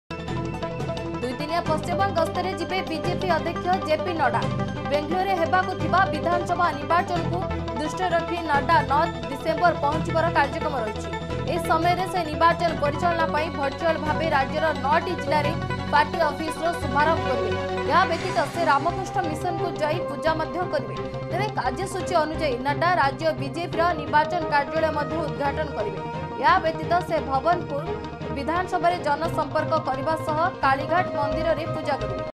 ୨ ଦିନଆ ପଶ୍ଚିମବଙ୍ଗ ଗସ୍ତରେ ଯିବେ ଜେପି ନଡ୍ଡା
୨ ଦିନଆ ପଶ୍ଚିମବଙ୍ଗ ଗସ୍ତରେ ଯିବେ ଜେପି ନଡ୍ଡା